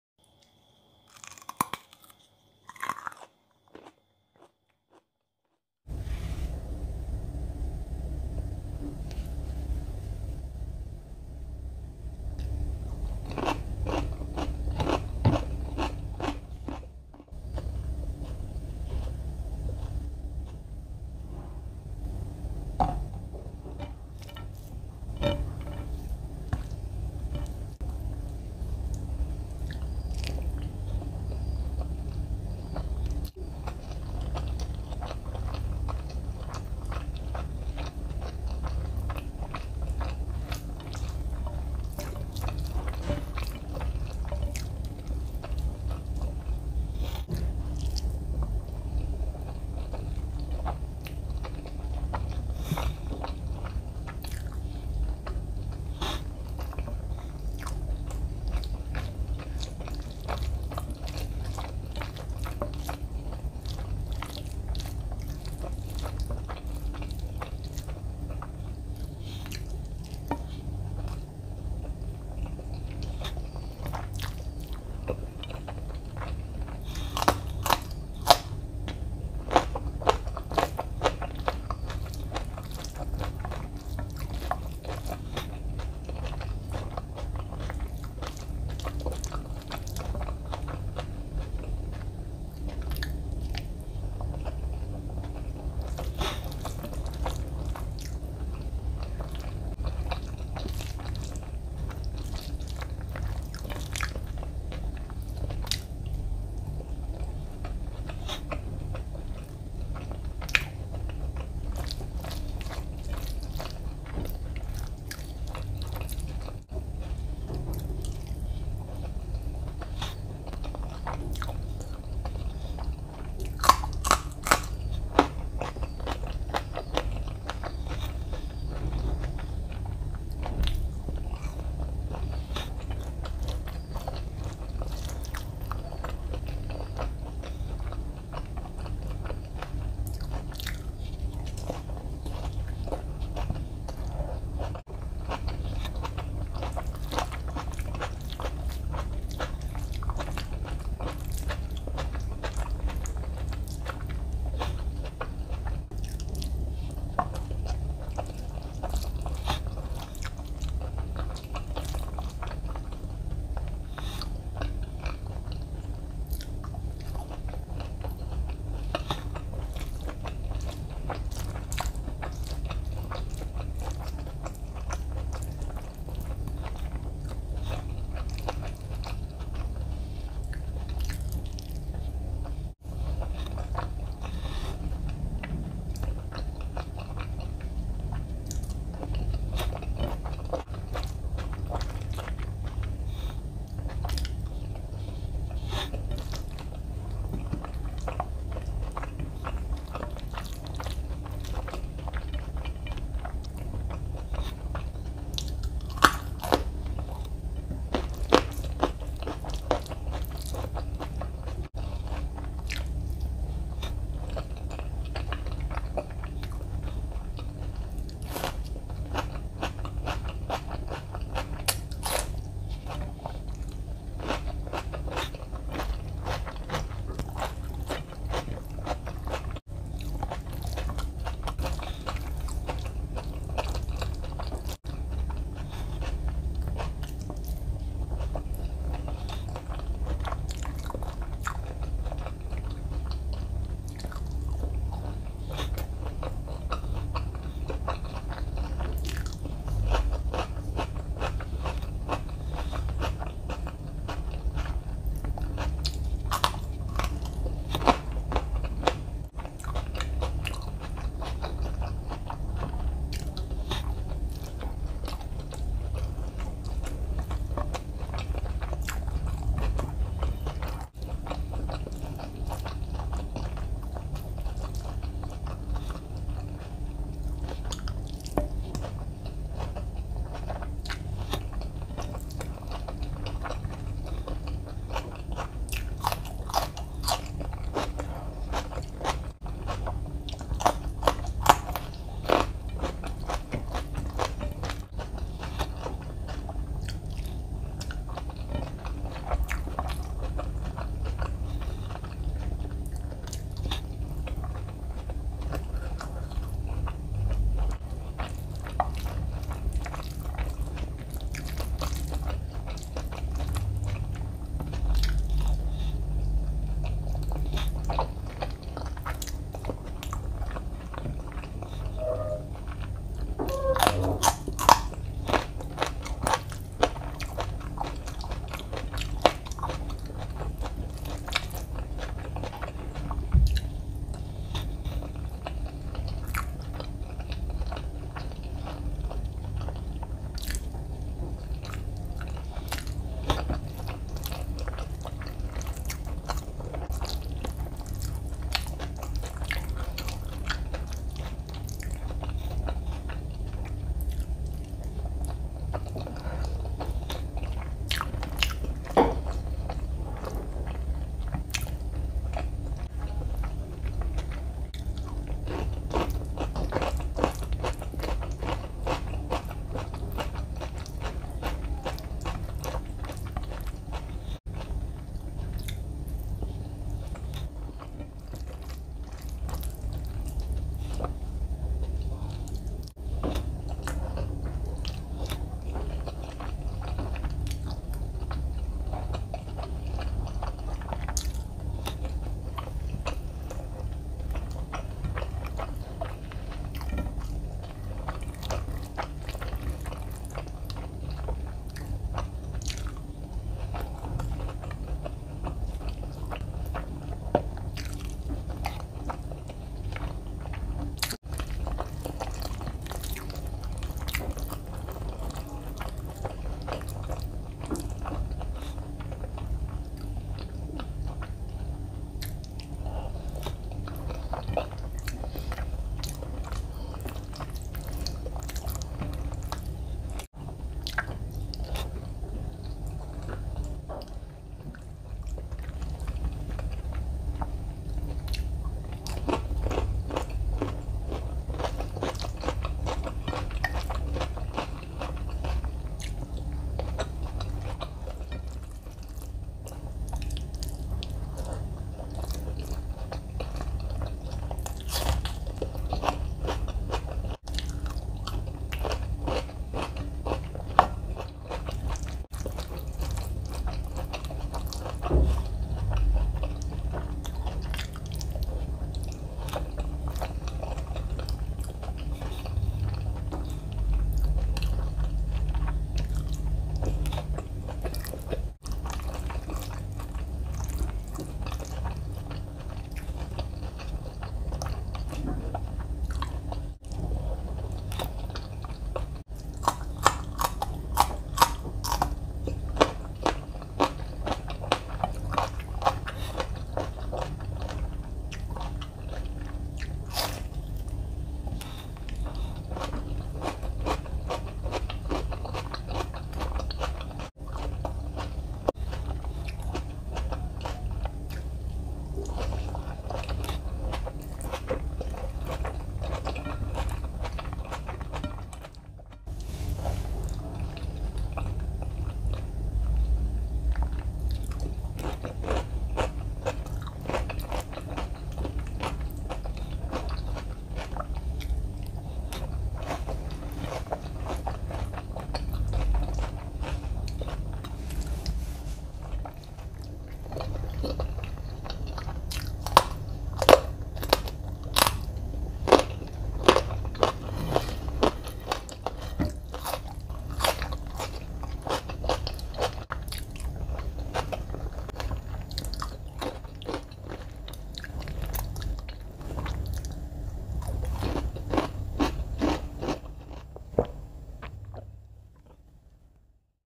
ASMR:INDIAN FOOD-SPICY RAJMA MASALA,KADHI PAKORE CURRY,MALAI PANEER,KADHI PAKORE ||ZAKIA-SPICY ASMR
ASMR:INDIAN FOOD-SPICY RAJMA MASALA,KADHI PAKORE CURRY,MALAI PANEER FRY,KADHI PAKORE, BASMATI RICE,CHILLI, RAW ONION, CUCUMBER, TOMATO(NO TALKING)EATING SOUND●BANGLADESHI FOOD EATING SHOW●||ZAKIA-SPICY ASMR